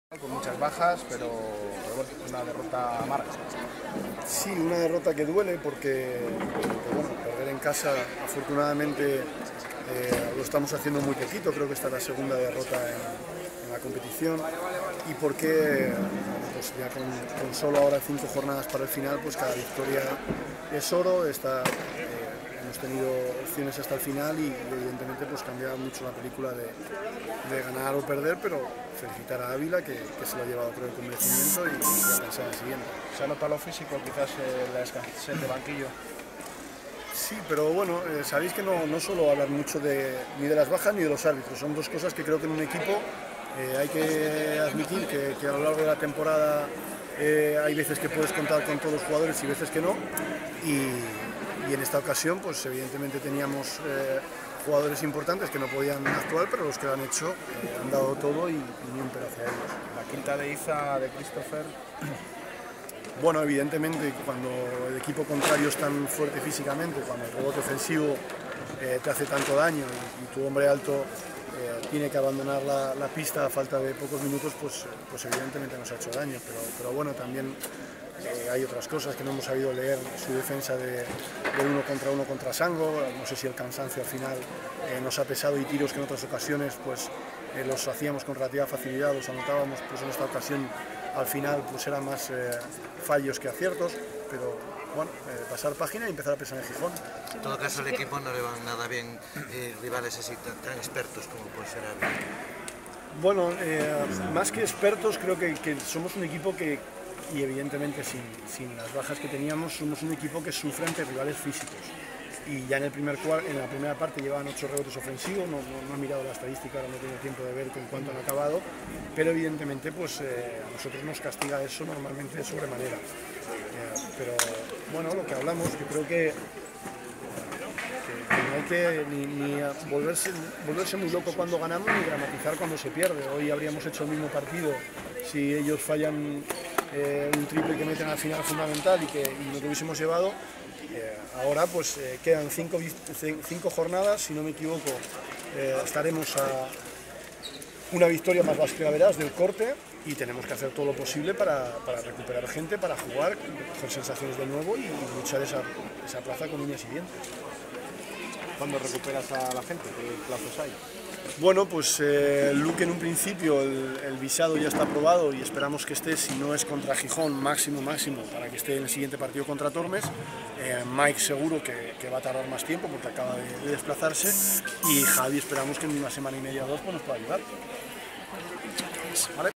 Postpartido J17: Saulo Hernandez
TITULARES | No era un partido fácil, ni fue el mejor partido. Derrota ante Óbila Club de Basket por 63 a 67. ¡Toca levantarse!

Próxima parada:
Círculo Gijón Baloncesto y Conocimiento
Sábado 19 (20:00)